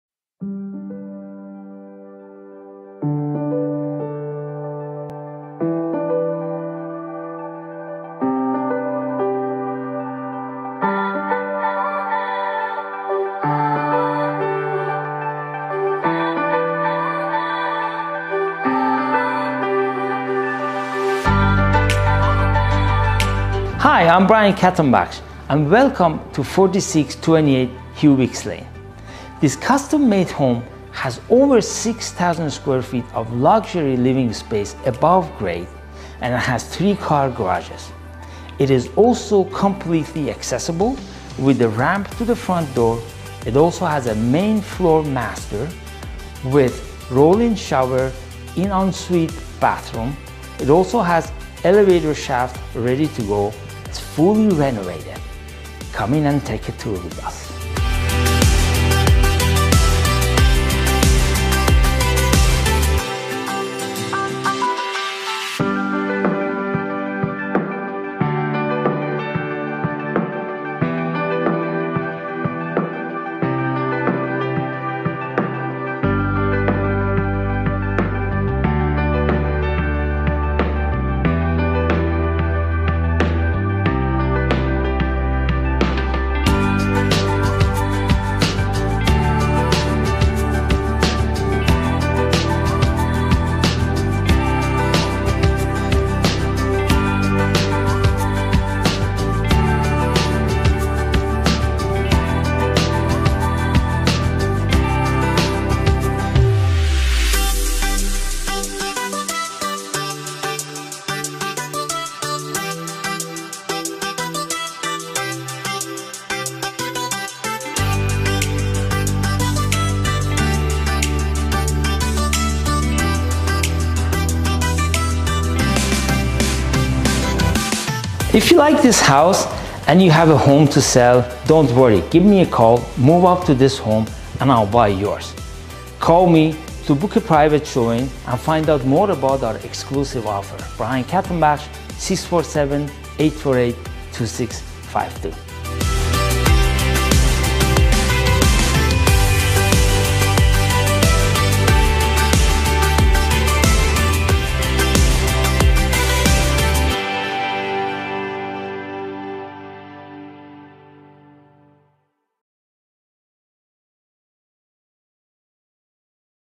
4628 Hewick's Lane, Mississauga, L5M 3L8 | Khatambakhsh Real Estate Team
4628 Hewick's Lane, Mississauga, L5M 3L8 | Brian Khatambakhsh Real Estate Team

🏡Buy this home and we’ll buy yours! Call for details.

A beautiful detached house in Mississauga:
🏡4628 Hewick's Lane🏡
✅Over 6,000 Sqft Ravine Lot⠀
✅Fully-Renovated in 2021 ⠀
✅Gourmet Kitchen & quartz Countertops
✅Main Floor Office & Master
✅Brand New Hardwood Floors & Appliances⠀
✅Great Neighbourhood⠀⠀

🏡  🏡  YOUR HOME SOLD GUARANTEED OR WE'LL BUY IT! some conditions apply